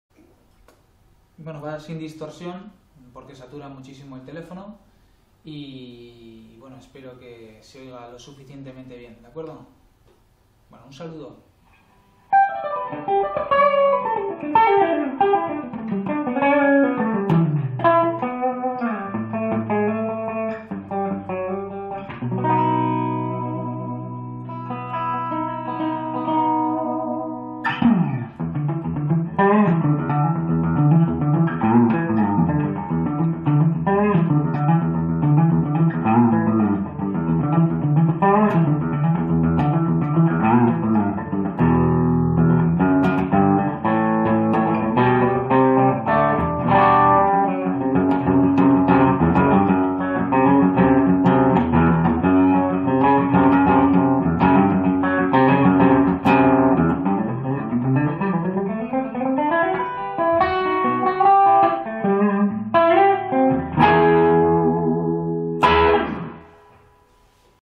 Respuesta 1
Respuesta al vídeo reto de guitarra de Yoxean.
No hay reglas e igual deberiamos haberlas especificado, si fraseos, rifs, rítmica, o sube-bajas, con o sin distorsión, etc. Bueno. Que sea lo que sea.